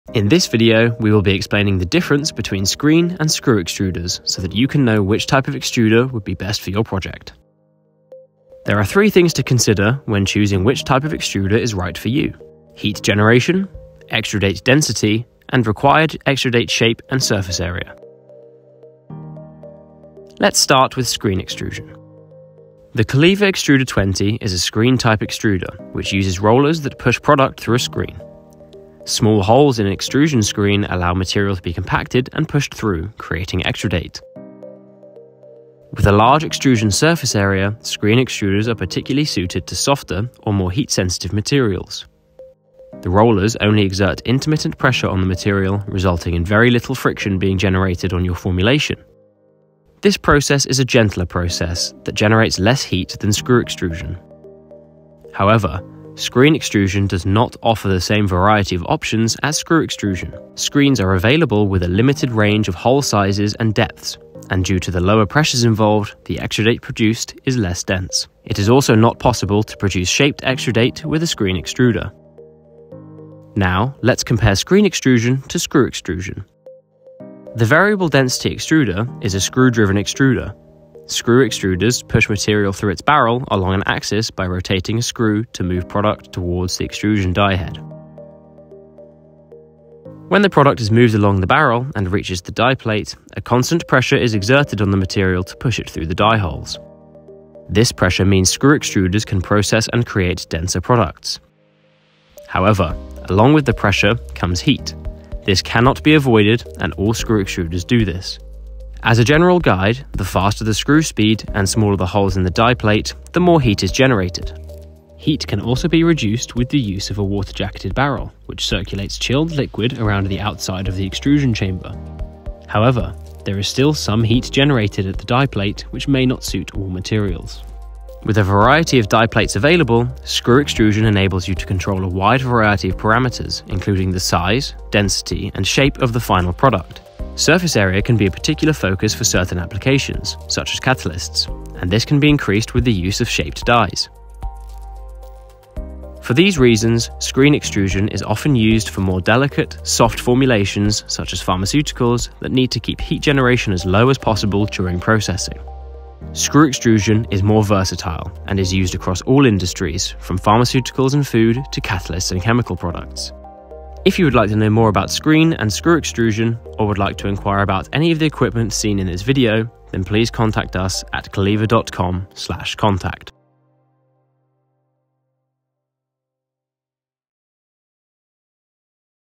Screen vs Screw Extrusion, Which is Best? | Wet Granulation
Which extruder should you choose if you have heat sensitive materials?

What if you need different shaped extrudate?

How about varying the density of your extrudes?

In this video we explore these topics, helping to clear up any questions you may have about whether screen or screw extrusion would be more suitable for your materials and projects.

Whether you're working on pharmaceutical pellets, catalysts, food formulations or any other product, choosing the right extruder can be instrumental to your project's success.